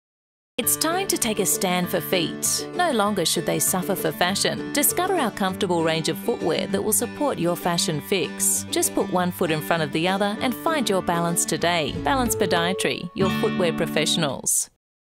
Balance Podiatry - Your footwear professionals
It’s time to take a stand for feet. No longer should they suffer for fashion. Discover our comfortable range of footwear that will support your fashion fix. With leading brands like Ziera, Frankie4, Merrell, Saucony, Naot and more, you're sure to find your perfect fit. Just put one foot in front of the other and find your balance today.